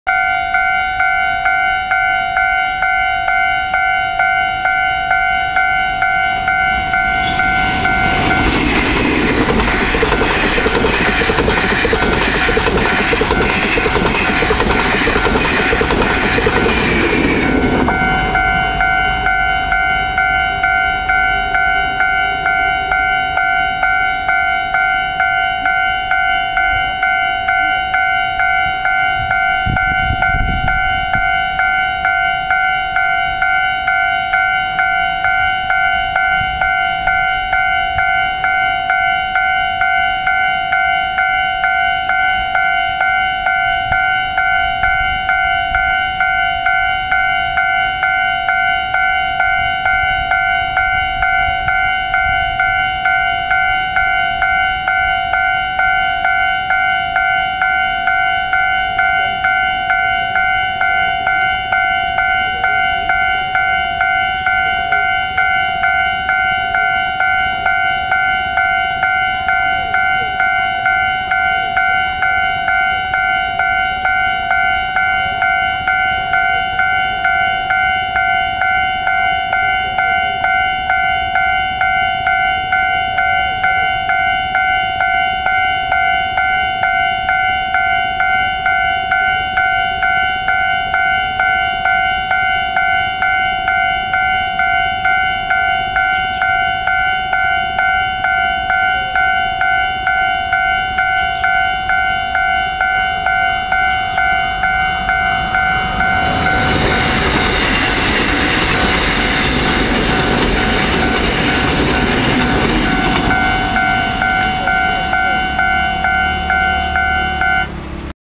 高雄岡山區大寮路平交道21
﹝縱貫線南段西側﹞北上PPE1000與南下8節EMU500